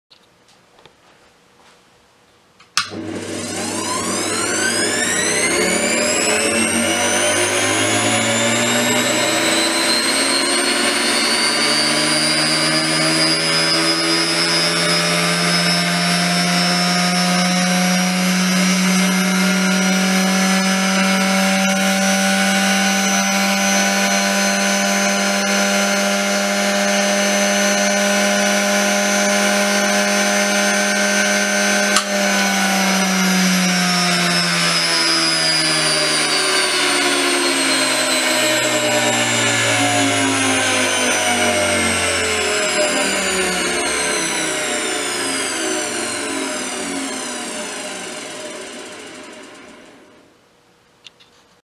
Luftwaffe Umfomer Ln 27381
Was delivered as a totally dirty and damaged Umformer.after cleaing,fixing some parts its woriking again.what a nice sound